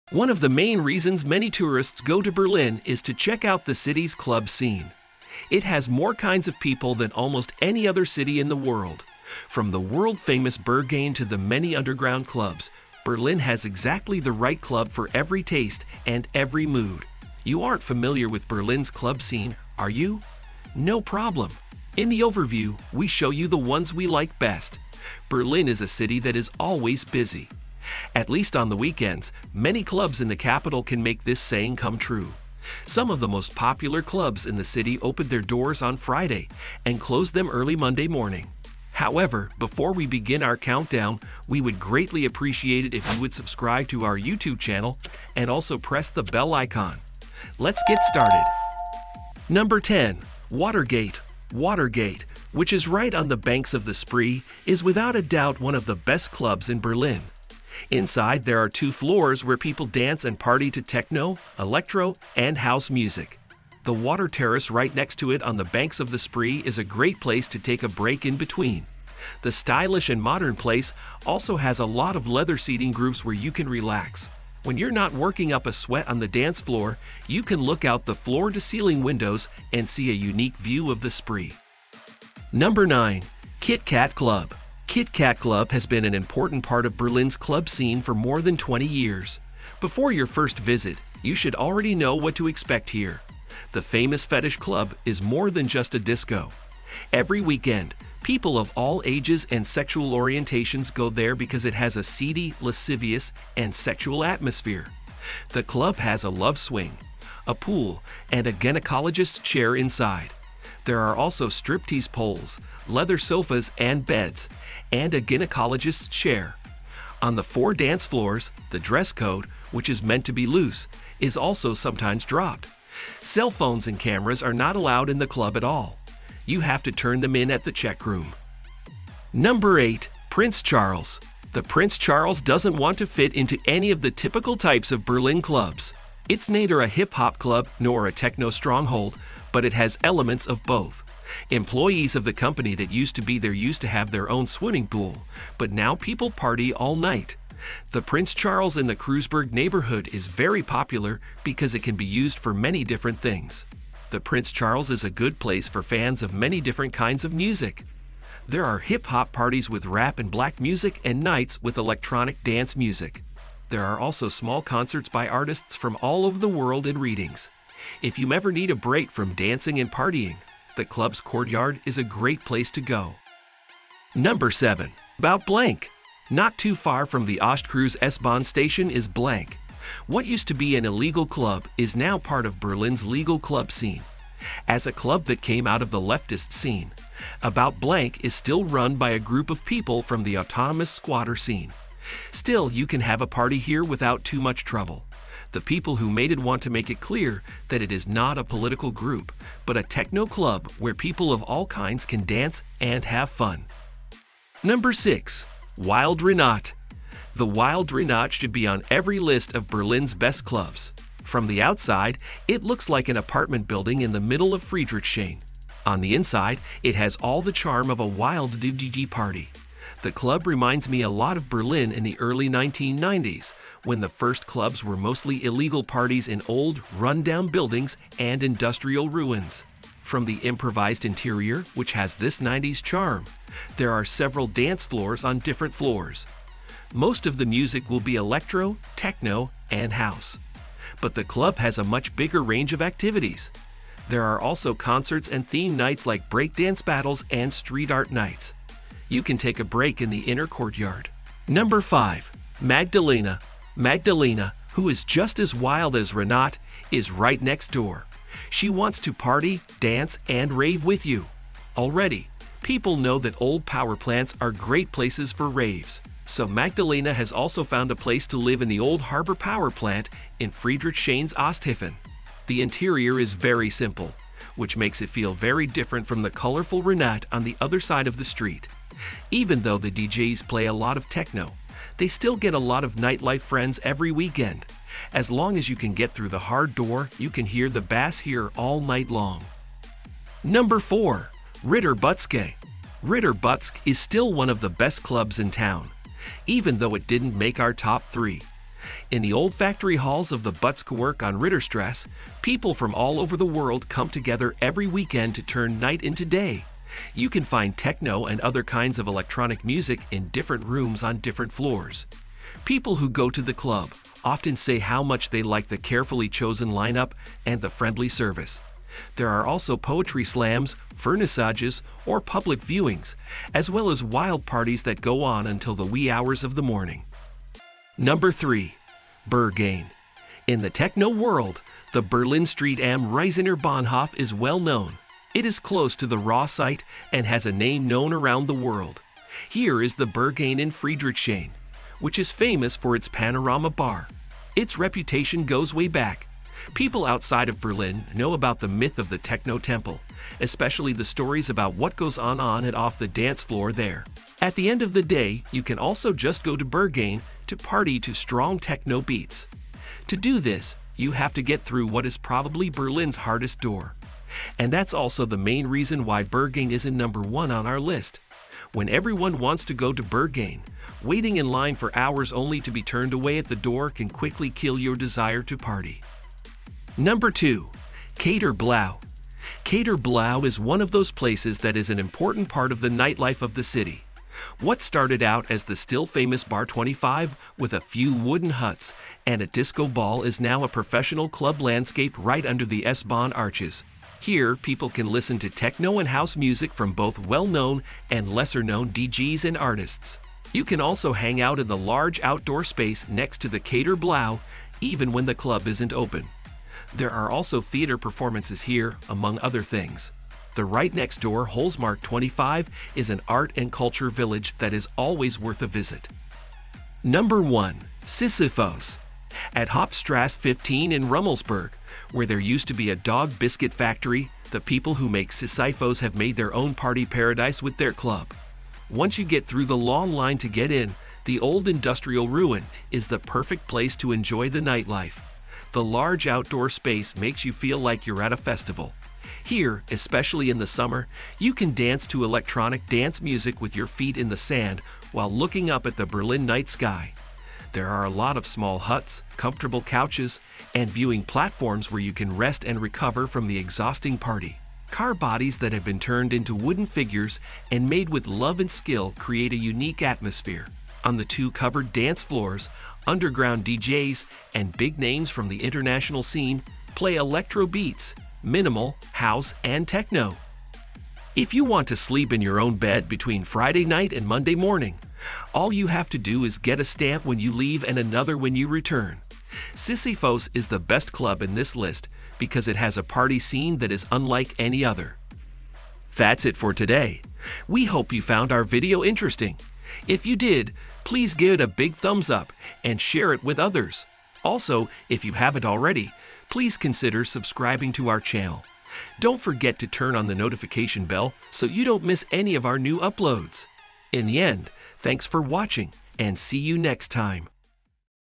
Top 10 best clubs in Berlin
In this video we are talking about :Top 10 best clubs in Berlin
One of the main reasons many tourists go to Berlin is to check out the city's club scene. It has more kinds of people than almost any other city in the world. From the world-famous Berghain to the many underground clubs, Berlin has exactly the right club for every taste and every mood. You aren't familiar with Berlin's club scene, are you? No problem! In the overview, we show you the ones we like best. Berlin is a city that is always busy. At least on the weekends, many clubs in the capital can make this saying come true. Some of the most popular clubs in the city open their doors on Friday and close them early Monday morning

Here is websites of clubs :

Number 10: Watergate
Address: Falckensteinstraße 49, 10997 Berlin

Number 9: KitKatClub
Address: Köpenicker Straße 76, Brückenstraße 1, 10179 Berlin

Number 8: Prince Charles
Address: Prinzenstraße 85F, 10969 Berlin

Number 7: :/about blank
Address: Markgrafendamm 24c, 10245 Berlin

Number 6: Wilde Renate
Address: Alt-Stralau 70, 10245 Berlin

Number 5: Magdalena
Address: Alt-Stralau 1-2, Berlin, Germany

Number 4: Ritter Butzke
Address: Ritterstraße 24-27, 10969 Berlin

Number 3: Berghain
Address: Am Wriezener bhf, 10243 Berlin

Number 2: Kater Blau
Address: Holzmarktstraße 25, 10243 Berlin

Number 1: Sisyphos
Address: Hauptstraße 15, 10317 Berlin

You are watching 🎥: Top 10 best clubs in Berlin